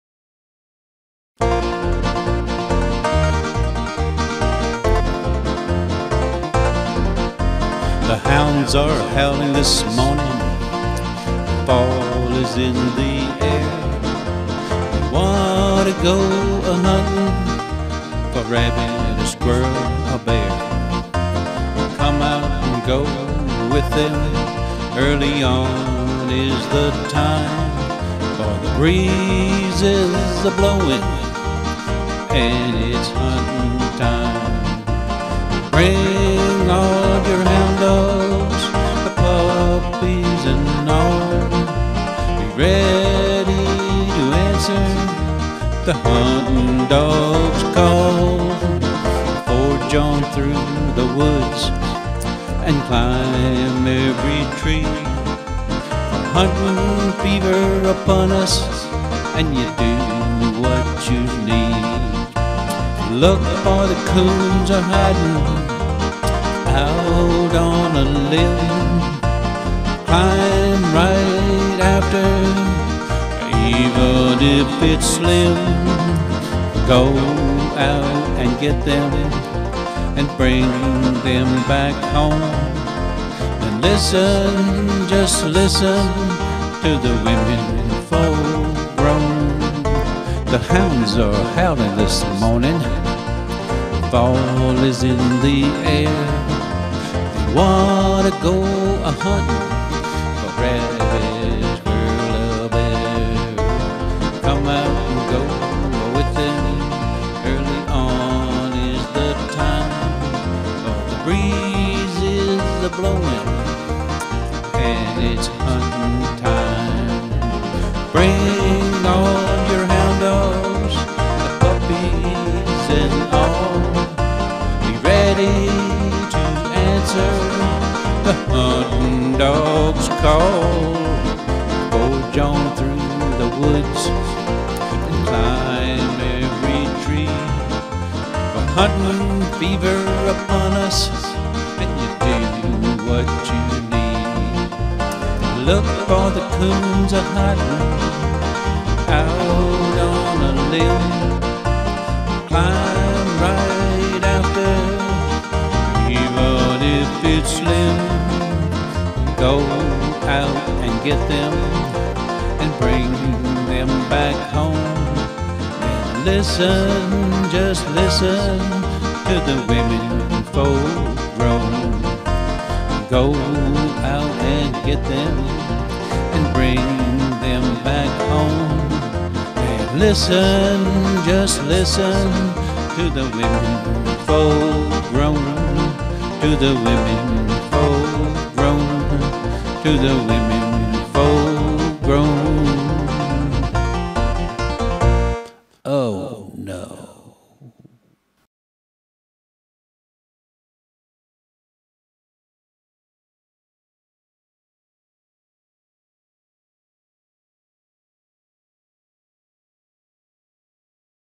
Hunting
A still picture video with original music about "Hunting"